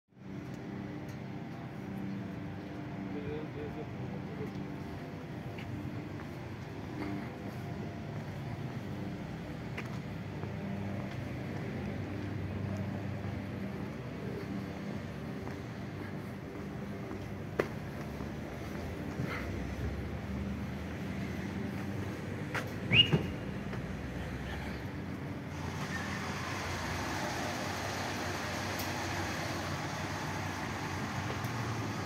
Olvidan cerrar el cajero después de su mantenimiento
Desde hace una semana esta abierto con las llaves de las gavetas pegadas el cajero en Complejo Industrial Américas.